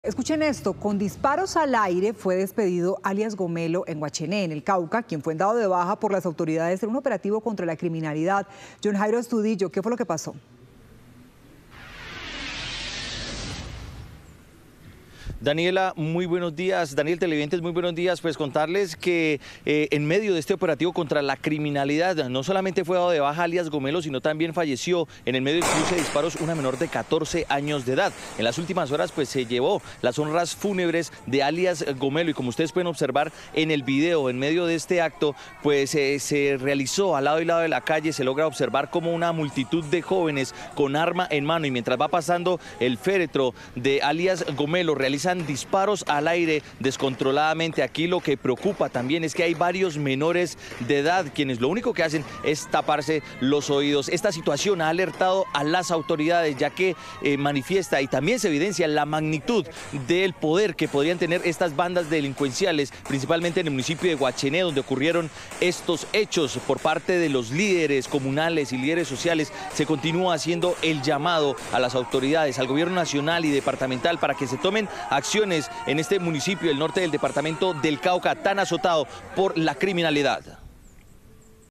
En medio de tiros despidieron a alias Gomelo, peligroso delincuente abatido en Cauca
En medio de operativos contra grupos armados y bandas delincuenciales en el Cauca fue dado de baja un delincuente conocido como alias Gomelo. Lo indignante del caso es que le hicieron un cortejo fúnebre por las calles, en medio de la ciudadanía y disparando ráfagas de armas de fuego al aire.

Varios jóvenes a lado y lado de la calle detonan incluso armas tipo escopeta al paso del féretro de alias Gomelo.